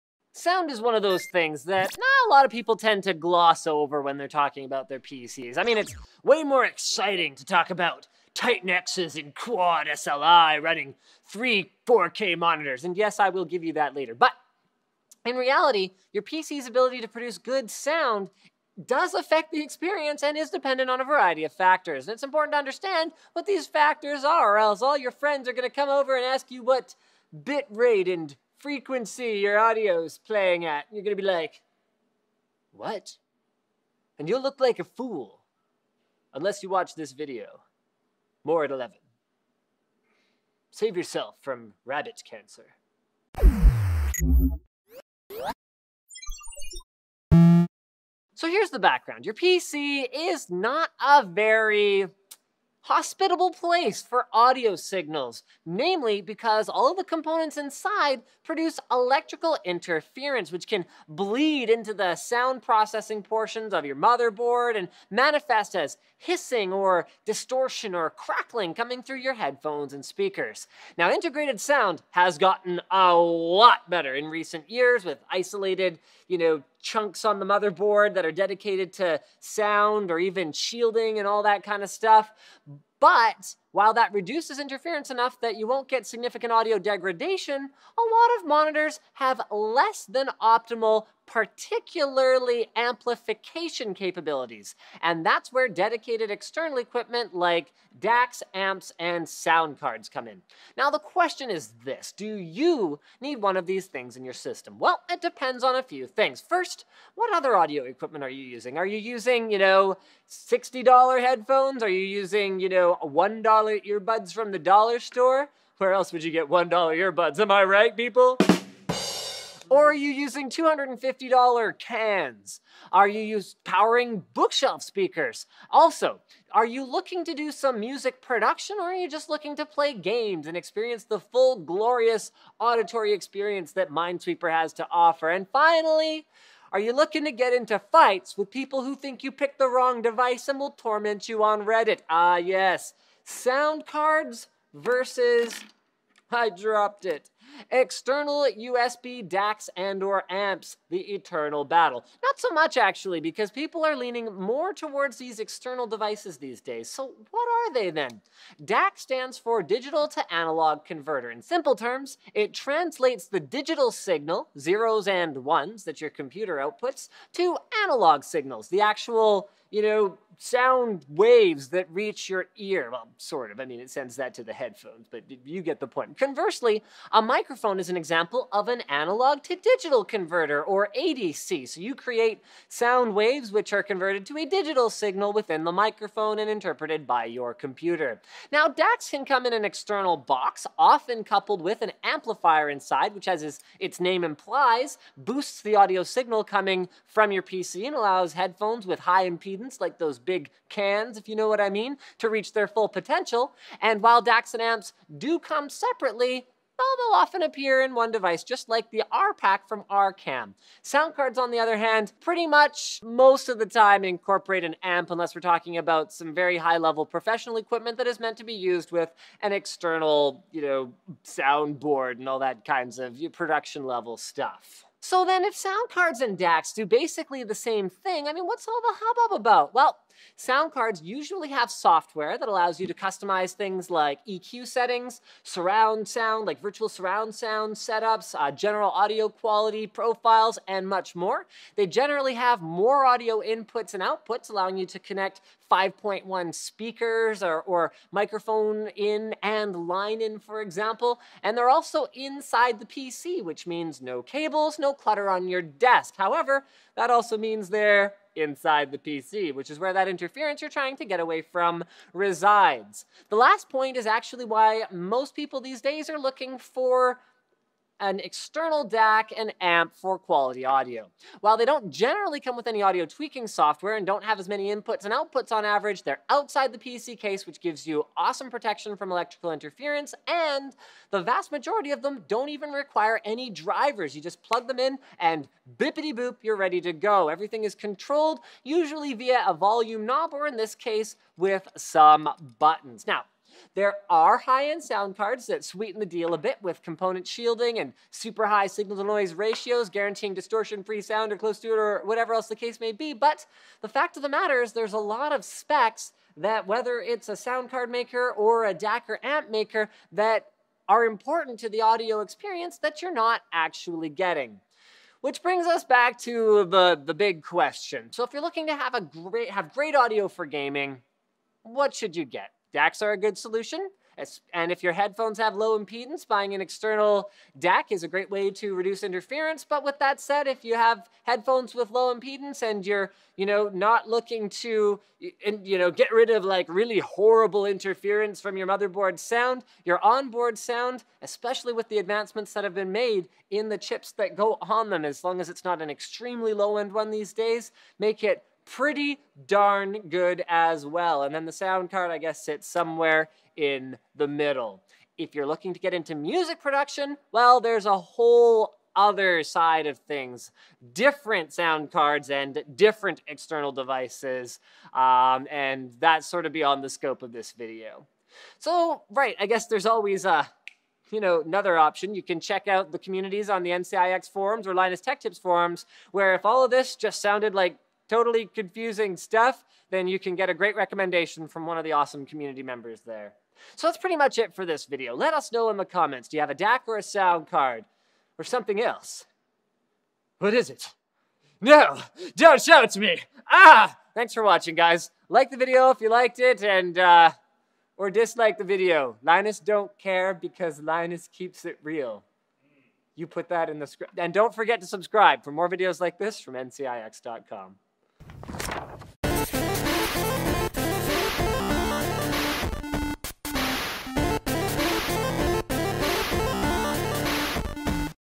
声卡到底需不需要？声卡又是什么鬼？ Why Sound Card?
15年前，配电脑的时候是要专门买声卡的。但是现在绝大多数的电脑都已经拥有的板载声卡，所以声卡可能是最早退化的电脑配件之一。不过这并不代表就没有人玩声卡了。今天就让Linus来说说声卡的那些事。